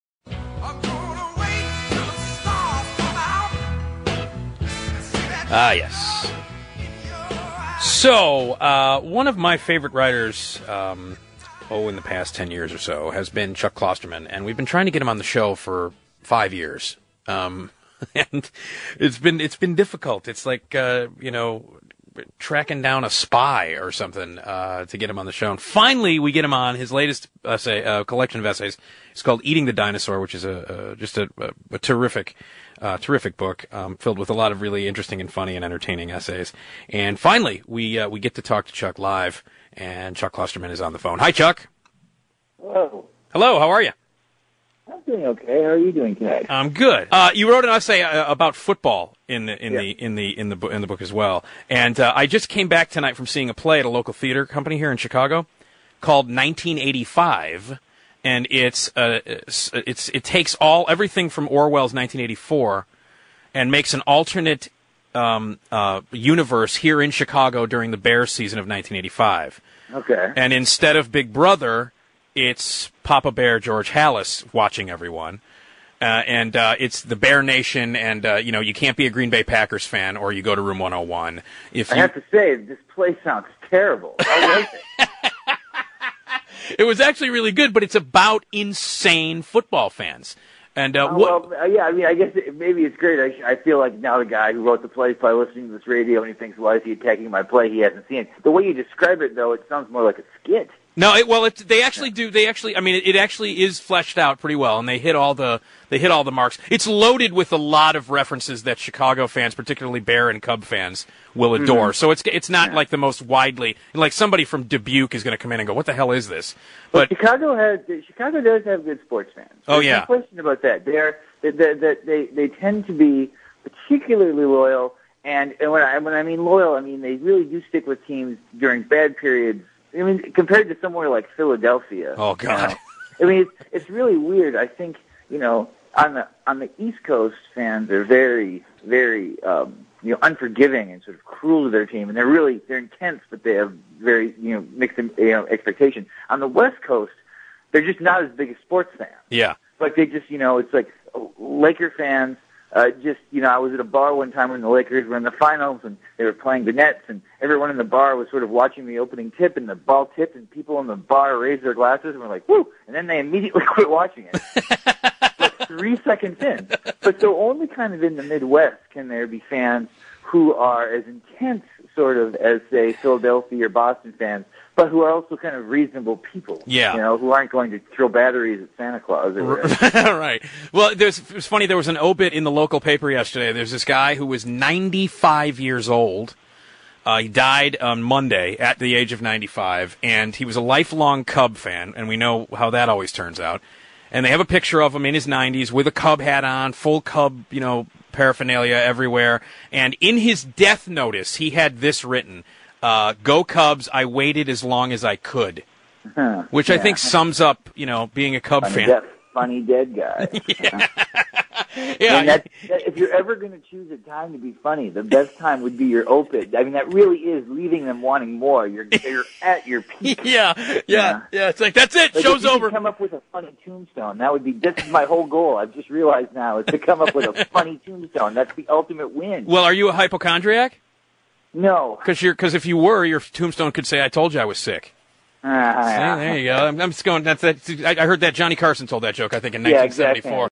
Digilio, Klosterman and Sports
Nick & Chuck Klosterman discuss Klosterman's new book Eating the Dinosaur, when Nick's trip to see Factory Theater's 1985 comes up, Klosterman has a knee-jerk reaction.  (Note: Klosterman subsequently admitted remorse to the playwright Chas Vrba for being quick to criticize the show sight unseen and Chas said he'd give Eating the Dinosaur a fair shake.)